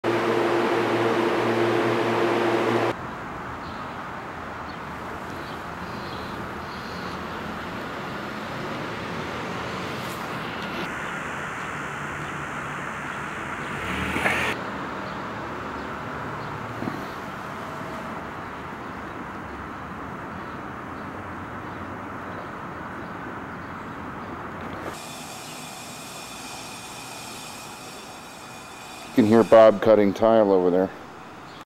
Kostas Closed and Southside bar addition under construction. 6/2/2003.
Tremont, Cleveland.